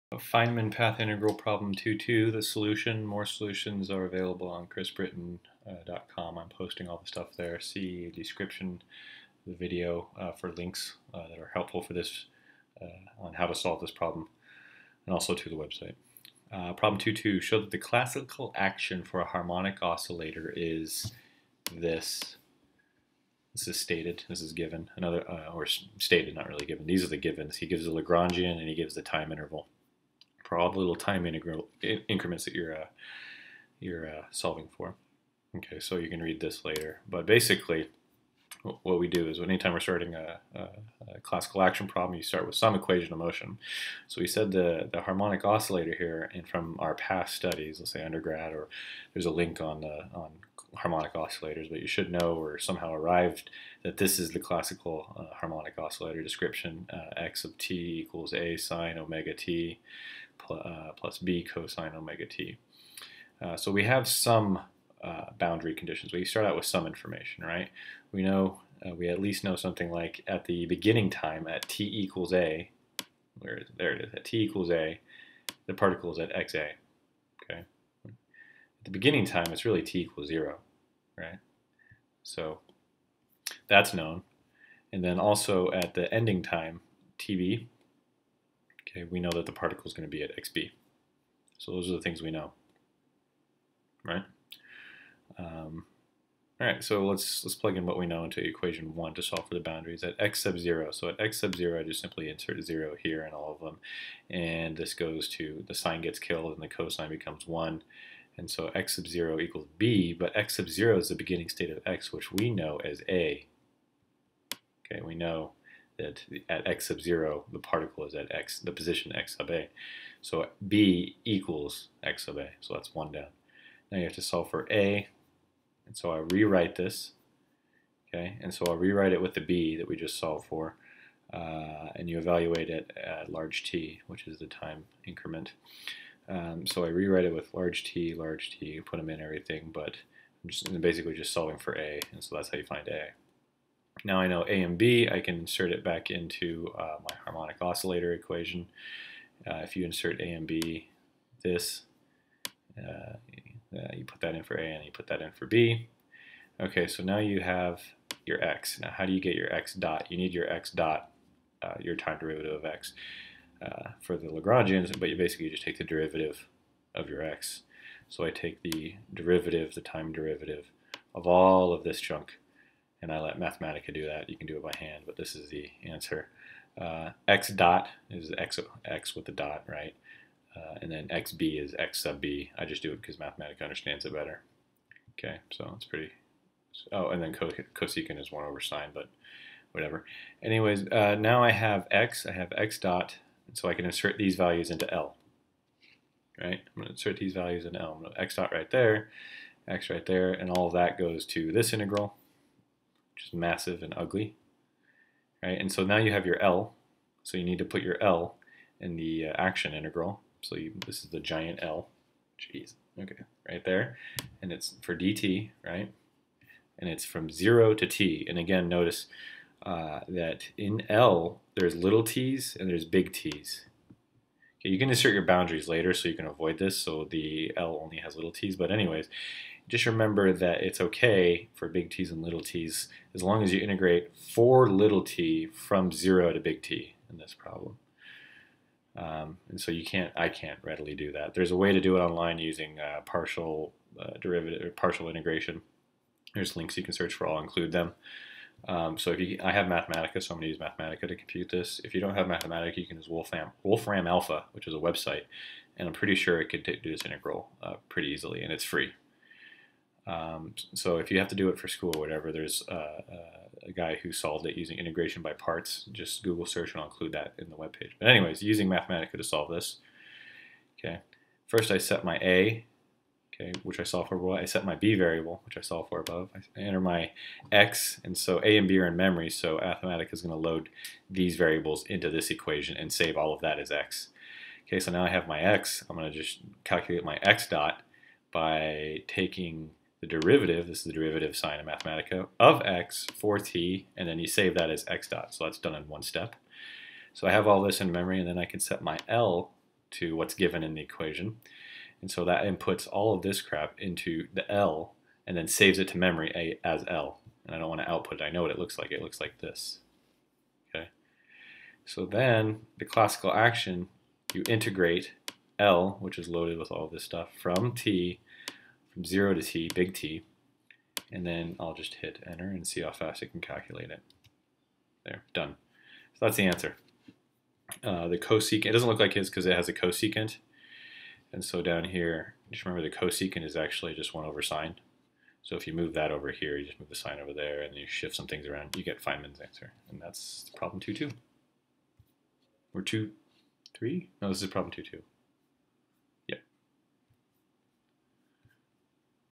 Feynman Quantum Path Integral Problem 2-2 Solution
Problem: Find the classical action of a harmonic oscillator. The Lagrangian is given.

The book I'm solving from is